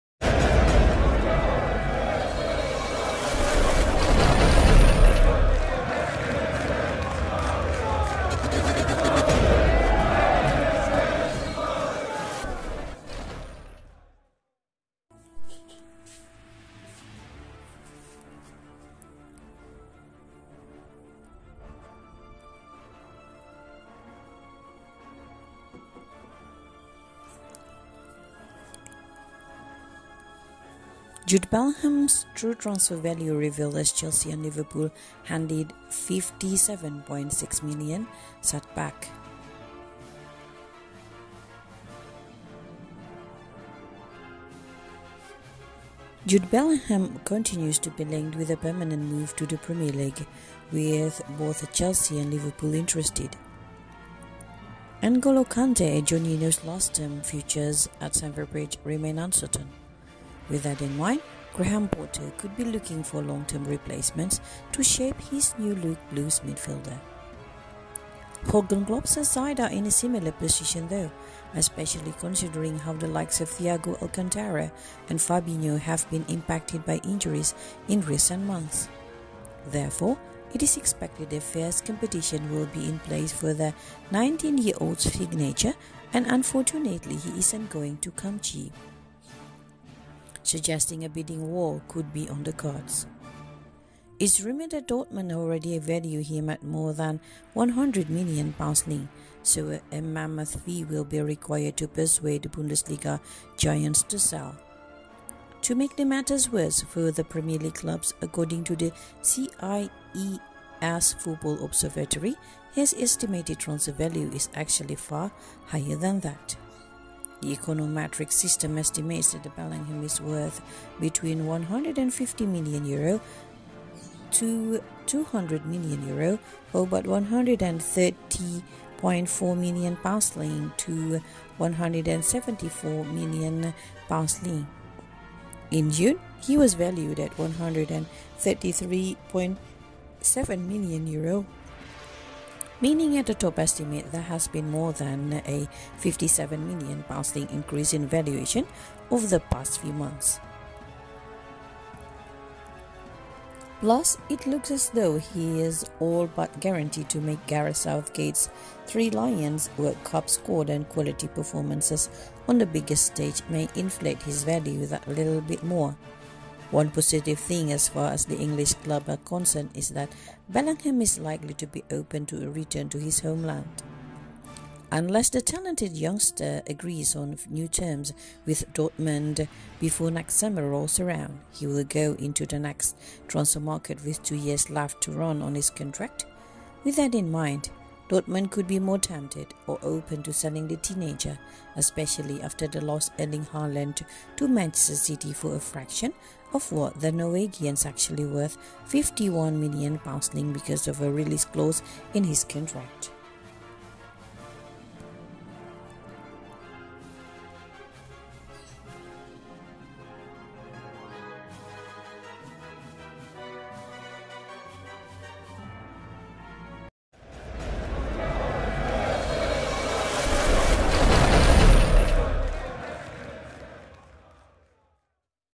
OFFICIAL: Bundesliga star true transfer value revealed as Chelsea handed£57.6m setback
Jude Bellingham's true transfer value revealed as Chelsea and Liverpool handed£57.6m setback

Jude Bellingham continues to be linked with a permanent move to the Premier League, with both Chelsea and Liverpool interested.

N'Golo Kante and Jorginho's long-term futures at Stamford Bridge remain uncertain. With that in mind, Graham Potter could be looking for long-term replacements to shape his new look Blues midfield.

Jurgen Klopp's side are in a similar position though, especially considering how the likes of Thiago Alcantara and Fabinho have been impacted by injuries in recent months. Therefore, it is expected that fierce competition will be in place for the 19-year-old's signature and unfortunately, he isn't going to come cheap - suggesting a bidding war could be on the cards.

It's rumoured that Dortmund already value him at more than £100million so a mammoth fee will be required to persuade the Bundesliga giants to sell. To make matters worse for the Premier League clubs, according to the CIES football observatory, his estimated transfer value is actually far higher than that.

The econometric system estimates that Bellingham is worth between €150million-€200million (£130.4million-£174million). In June, he was valued at €133.7million (£116.2million) meaning at the top estimate there has been more than a £57million increase in valuation over the past few months.

Plus, it looks as though he's all but guaranteed to make Gareth Southgate's Three Lions World Cup squad and quality performances on the biggest stage may inflate his value that little bit more. One positive thing as far as the English clubs are concerned is that Bellingham is likely to be open to a return to his homeland.

Unless the talented youngster agrees on new terms with Dortmund before next summer rolls around, he will go into the transfer market with two years left to run on his contract. With that in mind, Dortmund could be more tempted, or open, to selling the teenager, especially after they lost Erling Haaland to Manchester City for a fraction of what the Norwegians actually worth (£51million) because of a release clause in his contract.